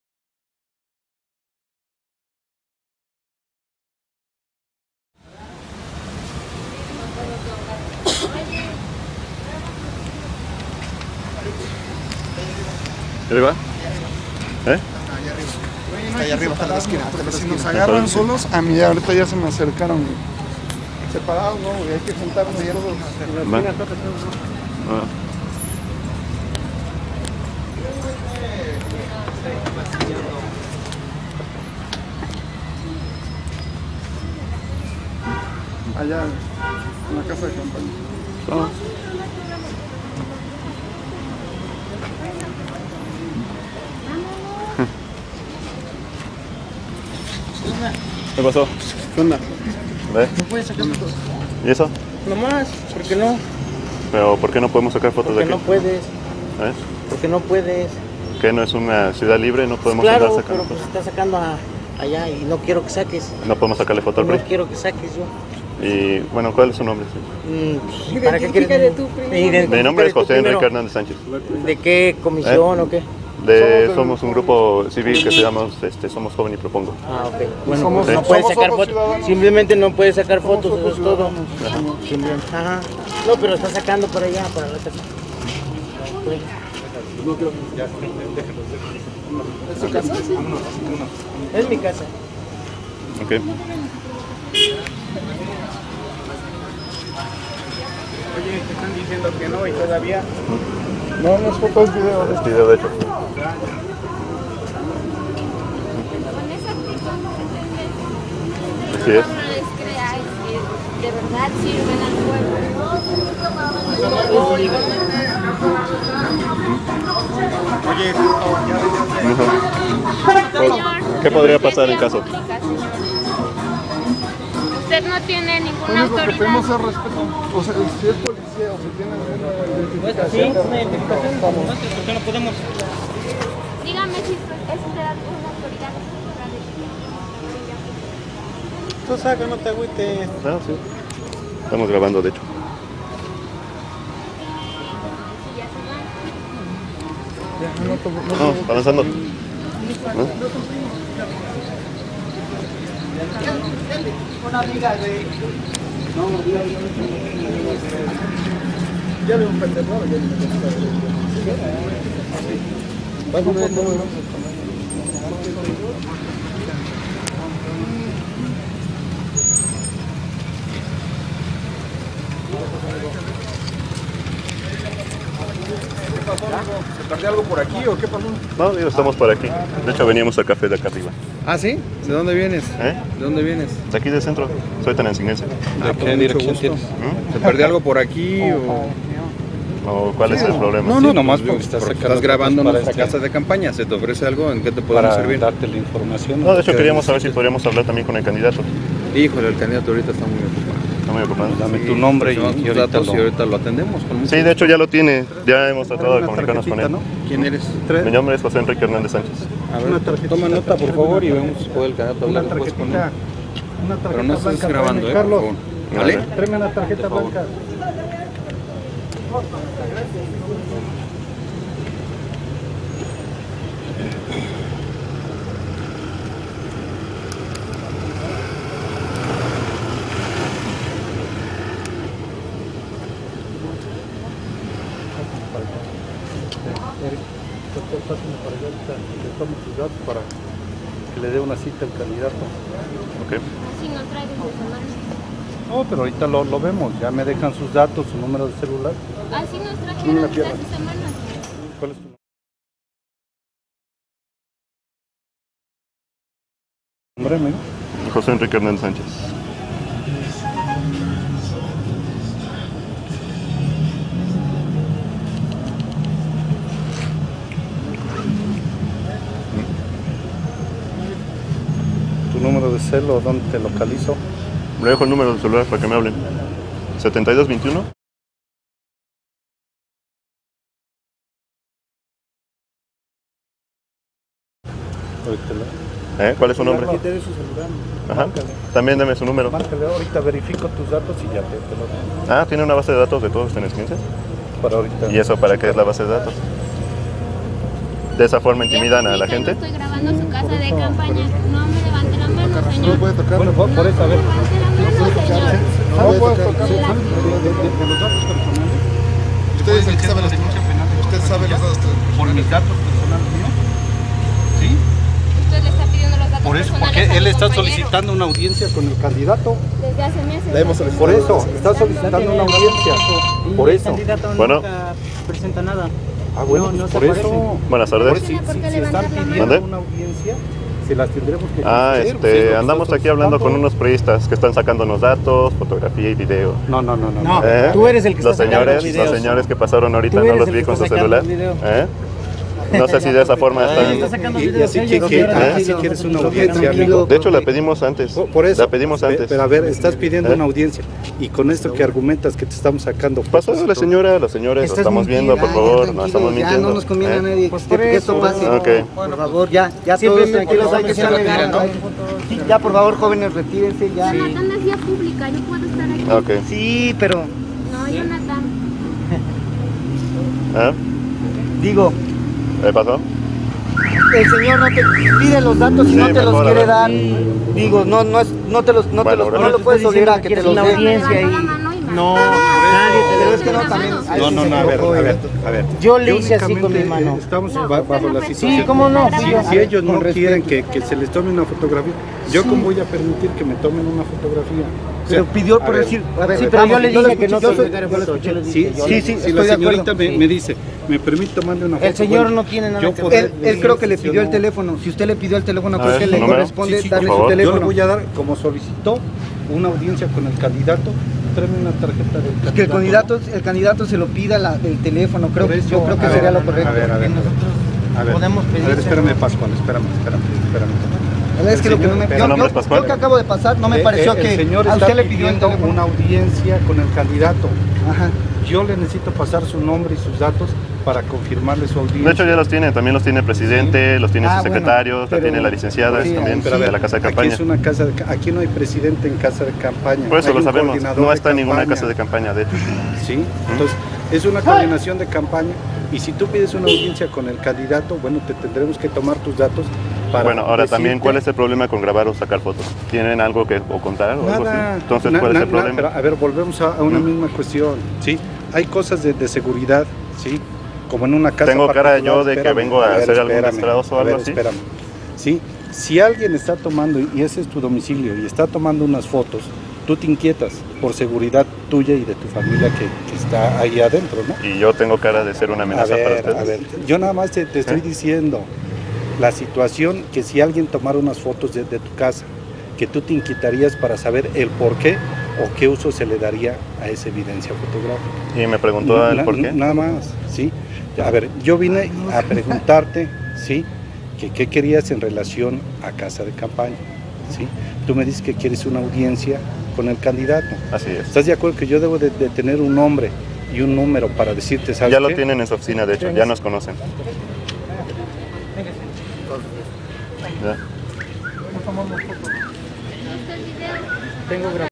Casa de campaña PRI intimidando a ciudadanos.
Published on Jul 1, 2012 by Soy Joven Y Propongo

Tratamos de graba la casa de campaña del candidato Roberto Sánchez Pompa, candidato del PRI, haciendo caso a una denuncia ciudadana, se nos dijo que el PRI estaba comprando votos cínicamente en el centro a la vista de todos, la reacción de ellos al vernos fue la esperada, ellos ya nos identifican como miembros de este movimiento por lo que no tardaron en detenernos e intentar intimidarnos